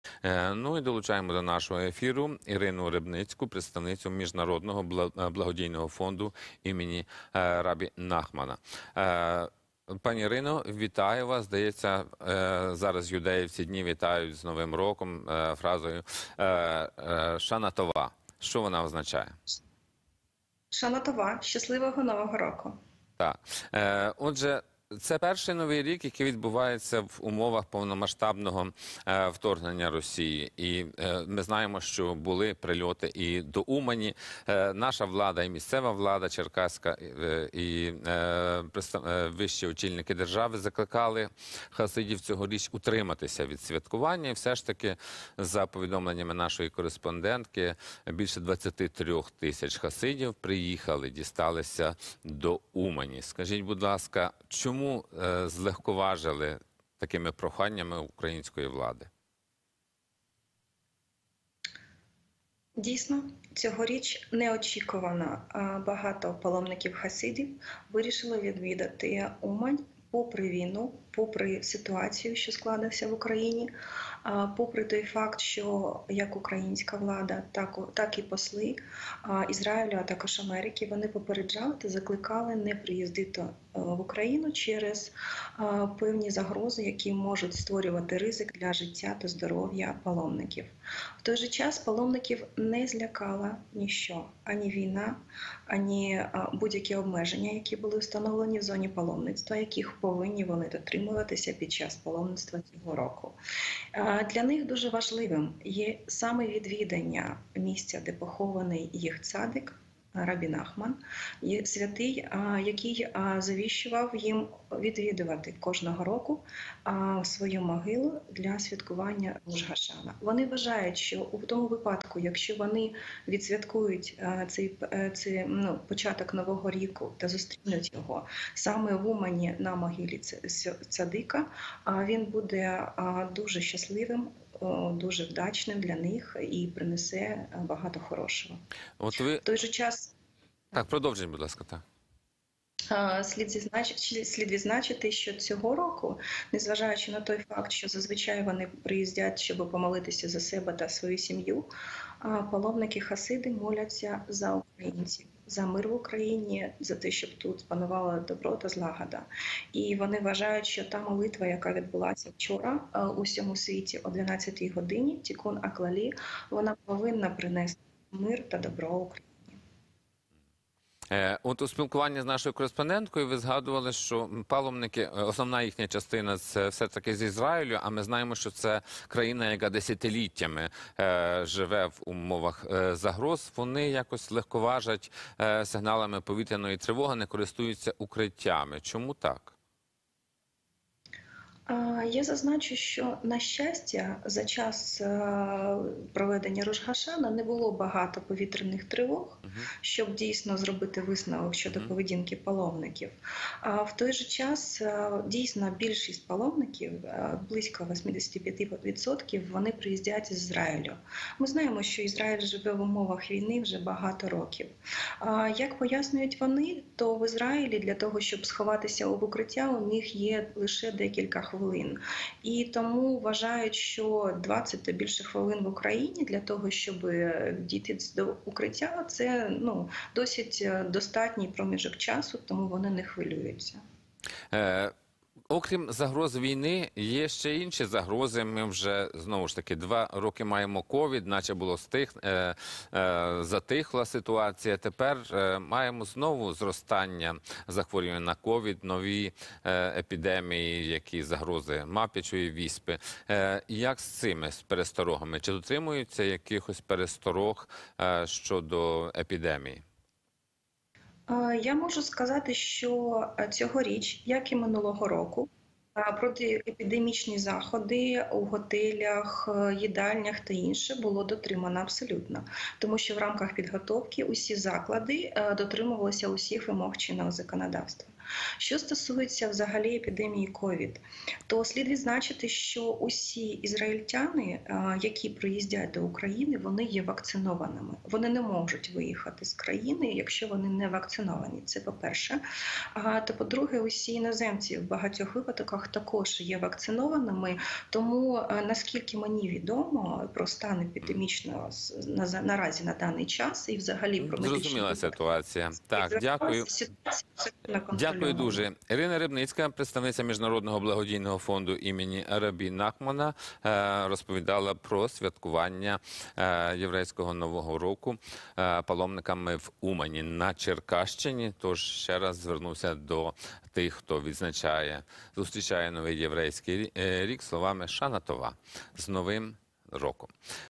Моляться за Україну і не бояться повітряних тривог: хасиди в Умані відзначають Рош га-Шана
Ірина Рибницька, представниця Міжнародного благодійного фонду імені Рабі Нахмана, розповіла в ефірі Суспільного, як проходить святкування Рош га-Шана в Умані. Паломників не злякали ані війна, ані певні обмеження в зоні паломництва. Для них дуже важливим є місце поховання цадика: вони вважають, що якщо зустрінуть новий рік на могилі цадика, то весь рік буде щасливим. Також Ірина Рибницька наголосила: цього року паломники-хасиди моляться за себе, свої родини і за мир в Україні.